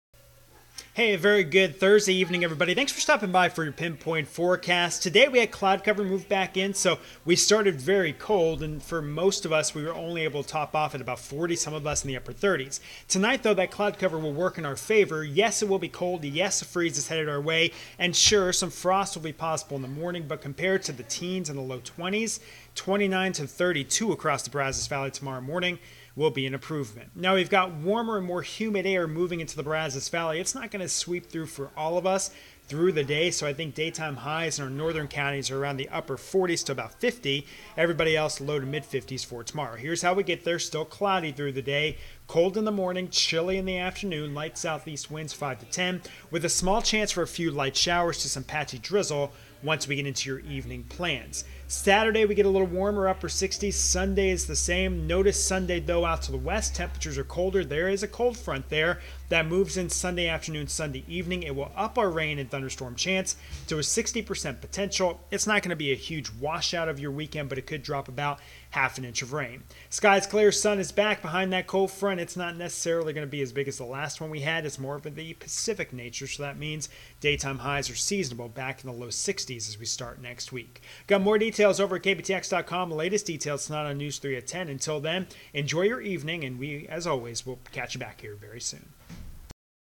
Thursday Evening PinPoint Weather Update 1.18.2018
Thursday Evening PinPoint Weather Update with Chief Meteorologist Shel Winkley